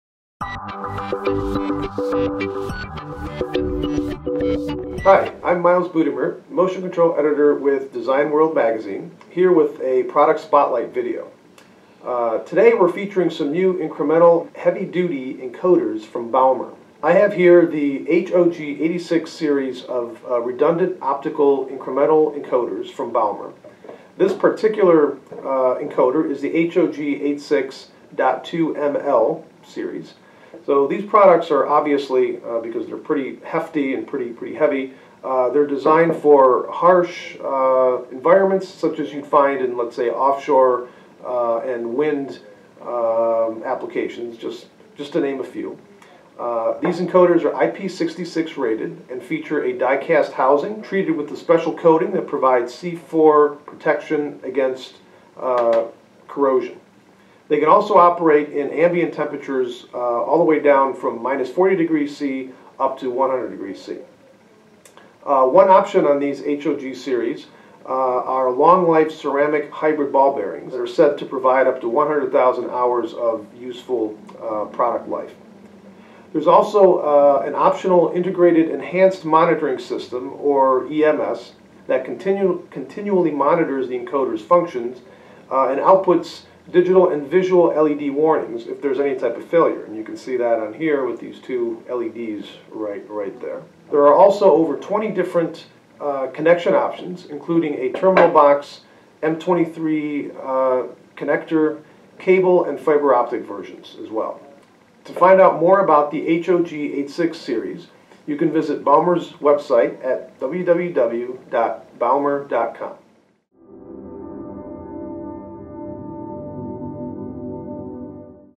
Product Showcase - Baumer - HOG 86
Design World product showcases the HOG 86 Incremental HeavyDuty Encoder by Baumer.